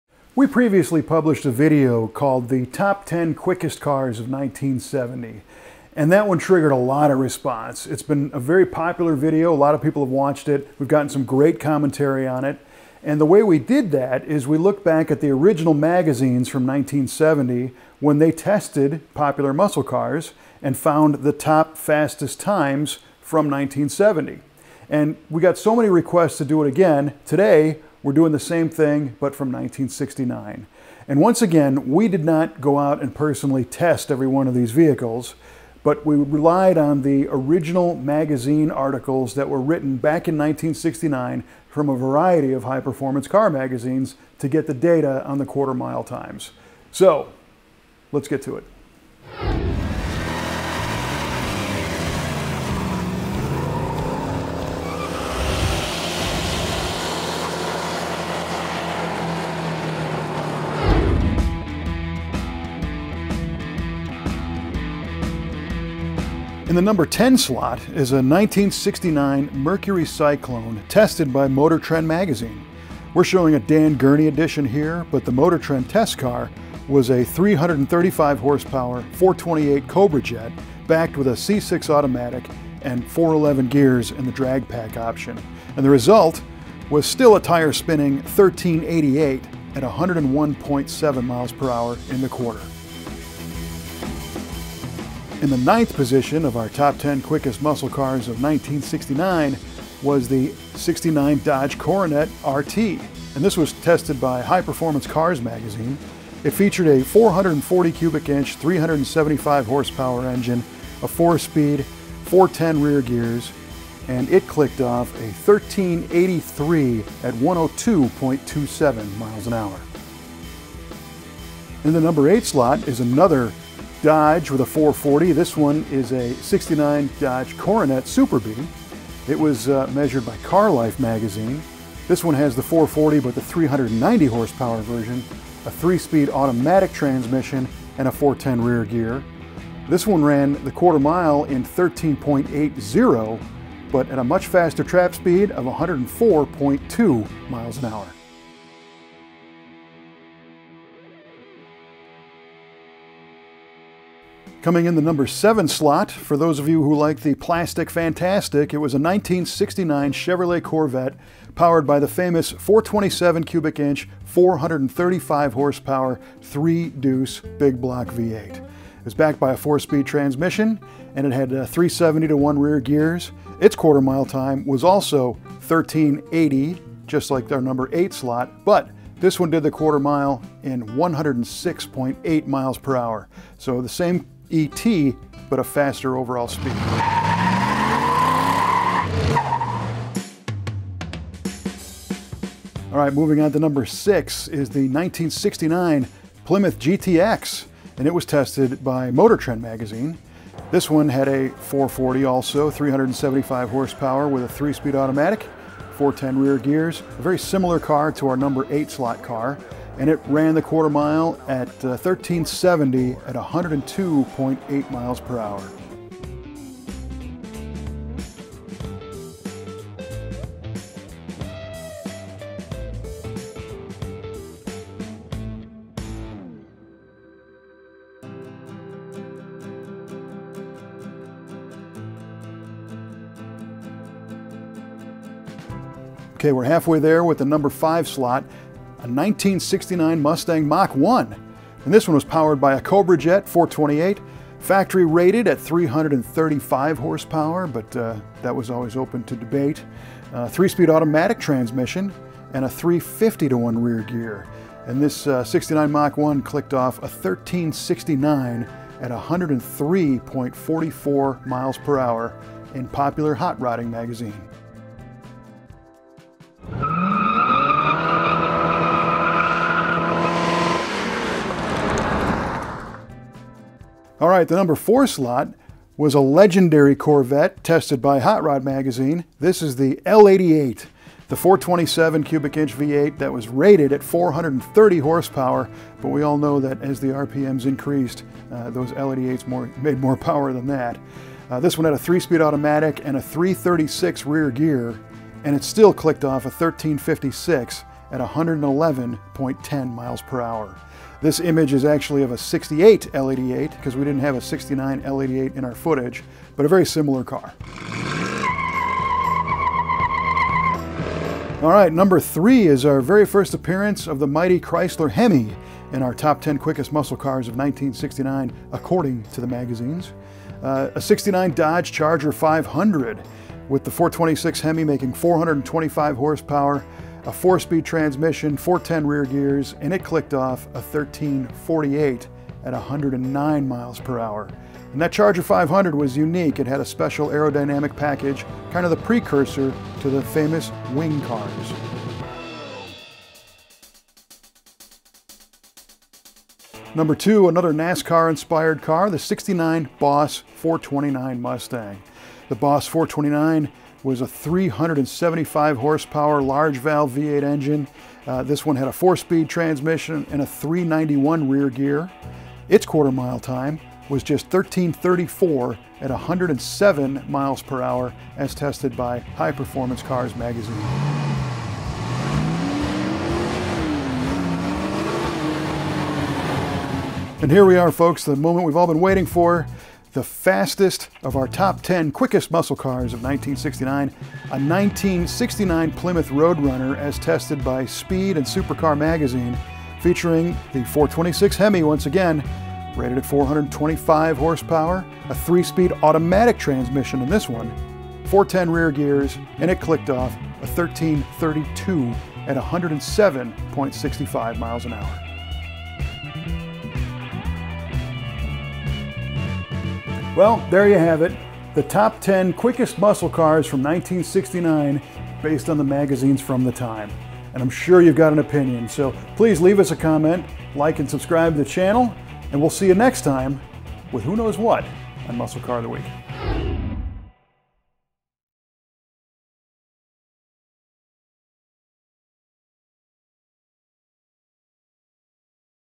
Top 10 Quickest Muscle Cars Of 1969 Muscle Car Of The Week Episode 373
We shared the top 10 quickest Muscle Cars of 1970, and it was so popular, Kevin Oeste is doing it again with more hot Muscle Cars from 1969.   We didn’t actually test these cars ourselves, but we’re utilizing the published test results from performance car magazines from the time.

10. 1969 Mercury Cyclone
9. 1969 Dodge Coronet R/T
8. 1969 Dodge Coronet Super Bee
7. 1969 Corvette
6. 1969 Plymouth GTX
5. 1969 Mustang Mach 1
4. 1969 Corvette L88
3. 1969 Dodge Charger 500
2. 1969 Mustang Boss 429
1. 1969 Plymouth Road Runner